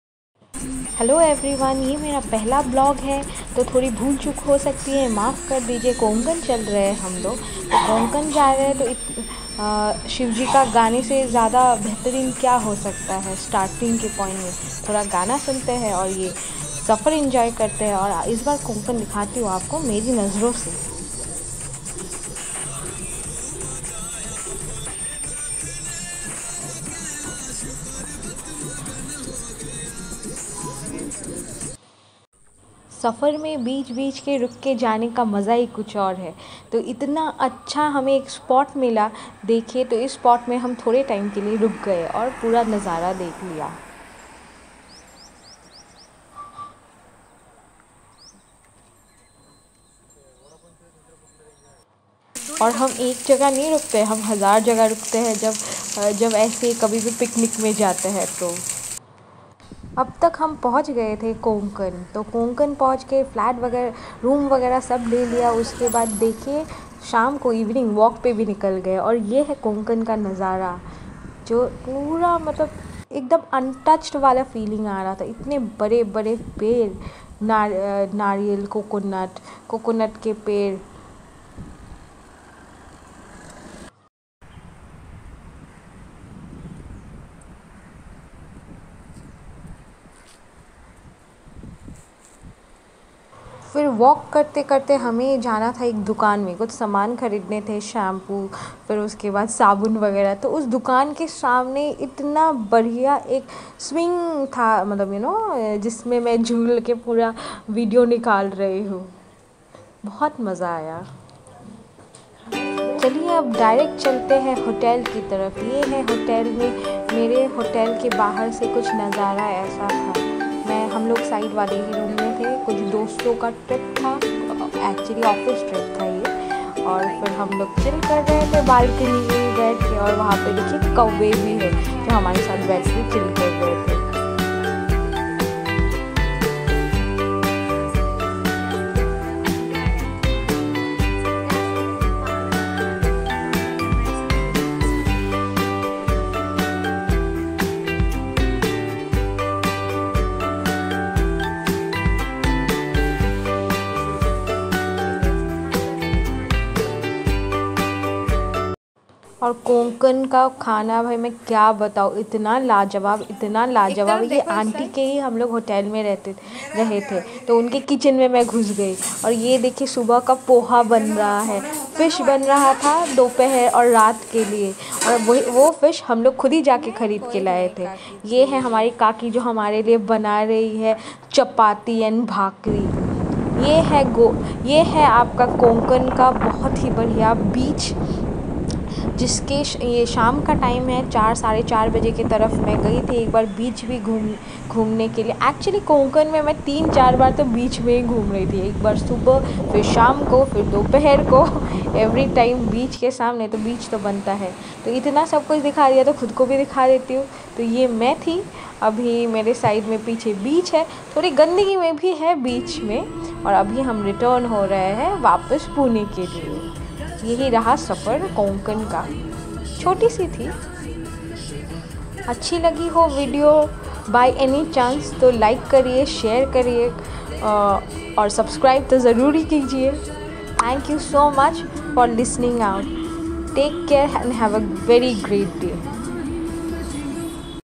Chaliye Chalte Hain Kokan ghar baithe baithe😀
A short video of Kokan, Exploring nature, food and wonderful people of India.